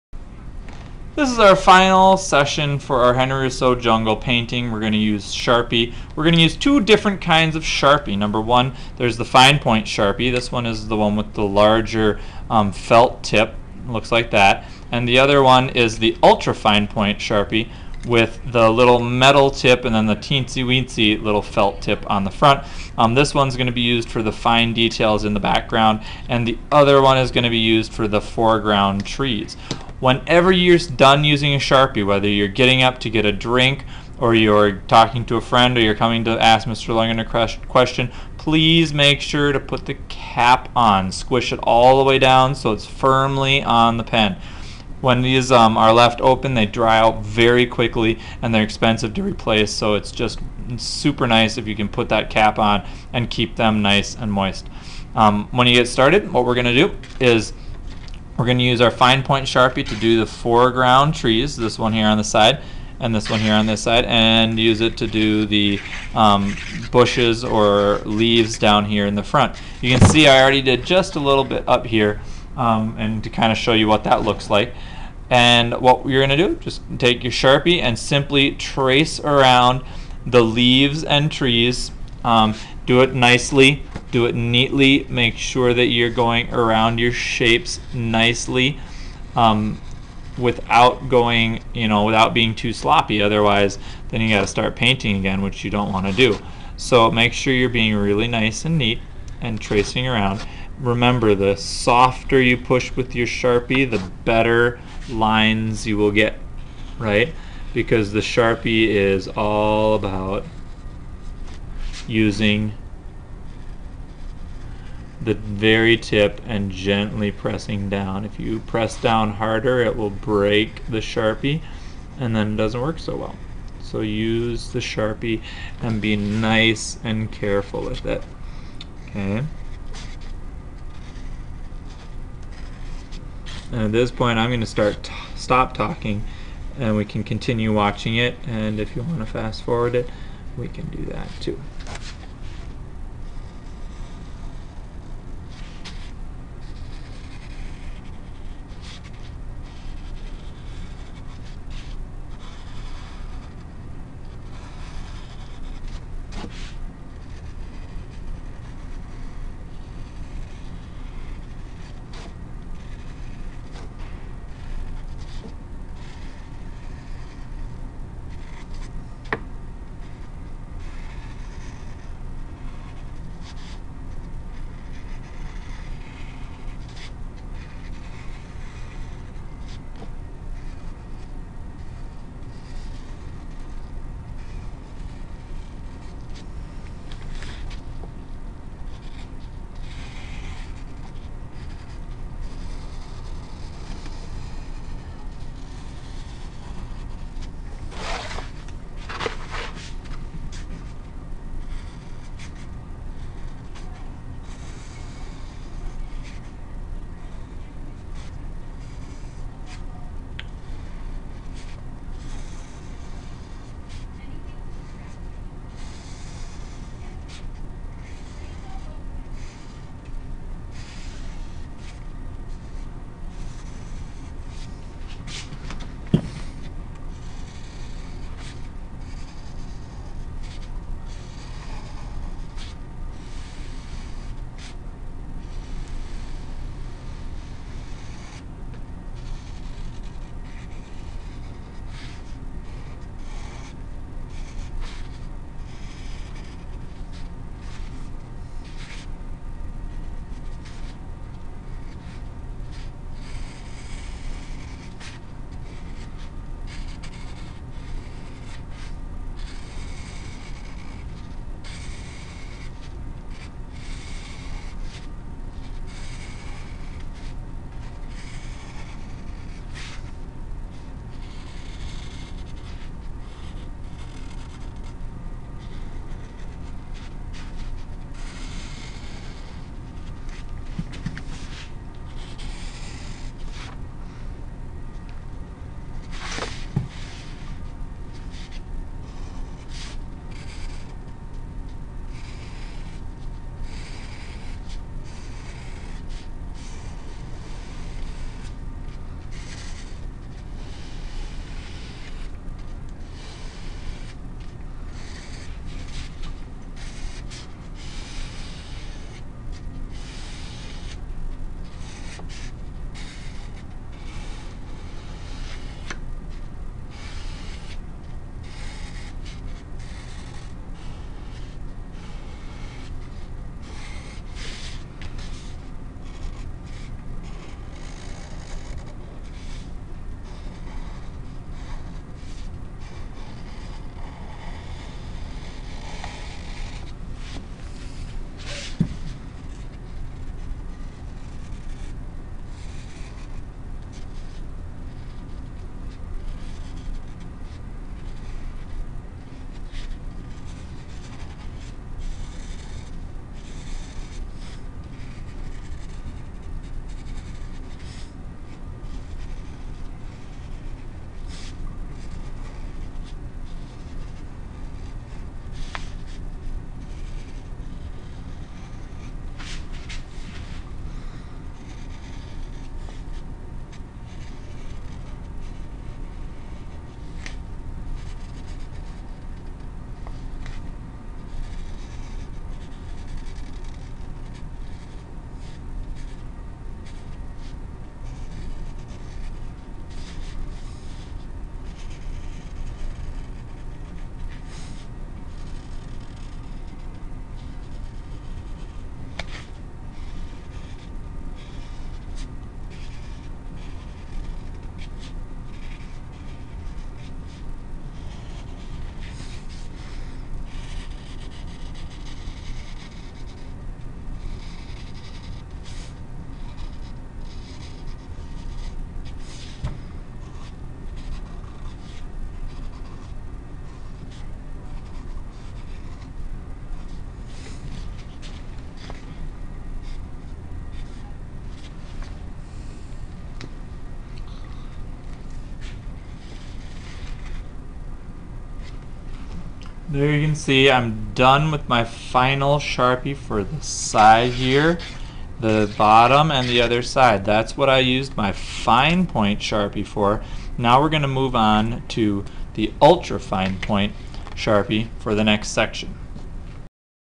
Sharpie follow up for Henri Rousseau Jungle Painting.MOV
In this video I silently finish my sharpie.